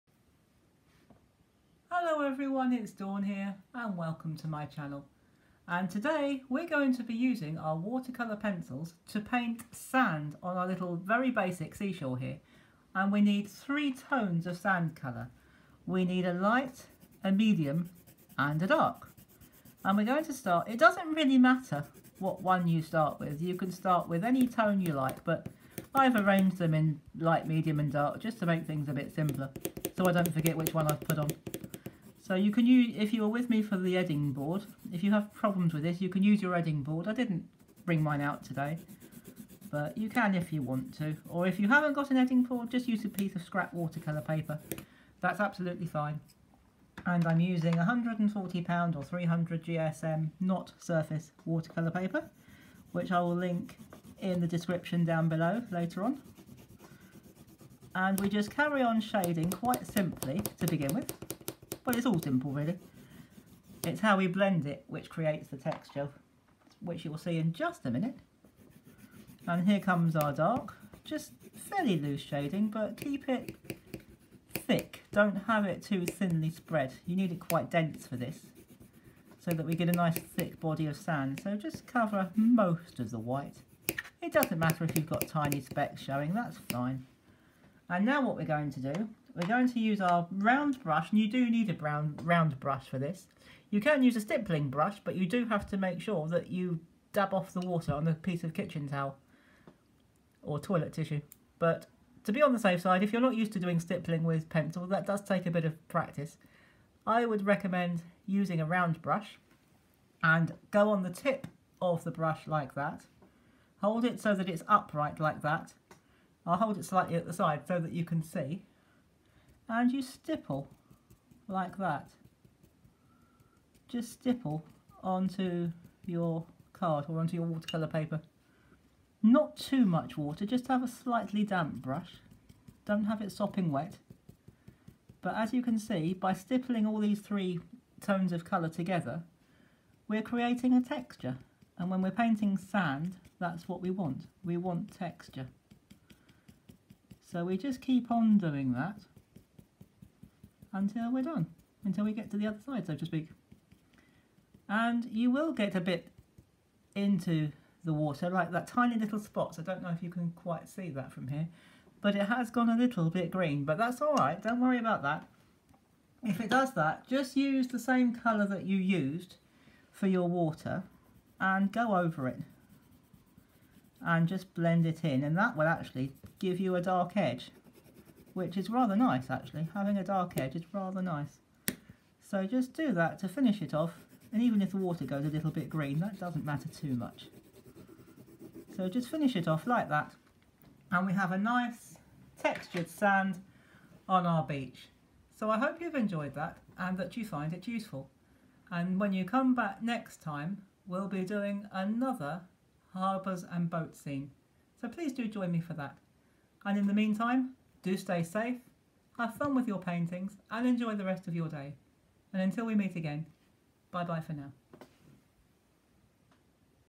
Paint sand with watercolour pencils
You need three tones of sand colour. A pale, a medium and a dark tone.
I used 140lb or 300gsm NOT surface watercolour paper.
NB A NOT surface means that it’s NOT smooth and it’s NOT rough. It had a texture that is in between .